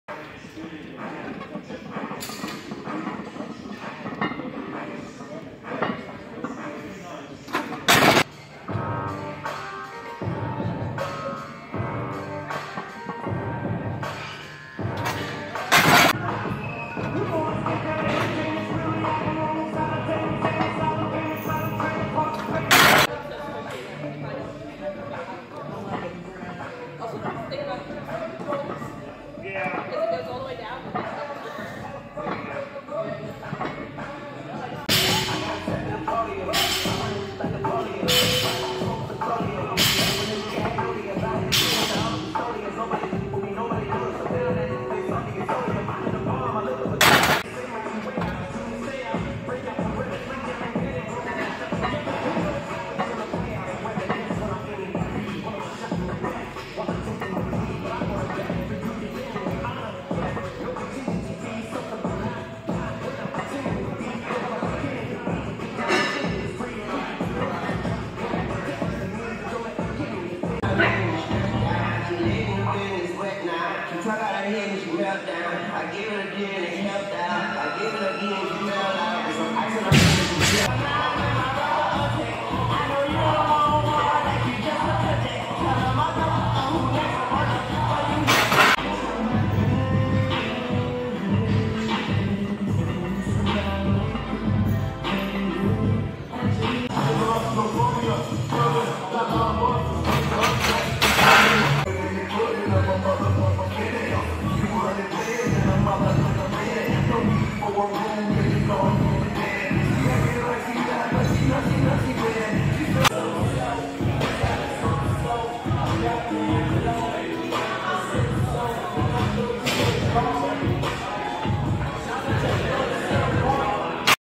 60 year old repping 500lbs raw on bench press. 520lbs x 2 ect. part of last weeks prep for meet.
here is a couple snips of my last 2 bench sessions. Turned 60 years old last month. still healing up from a partially torn tricep tendon. gotta keep grinding!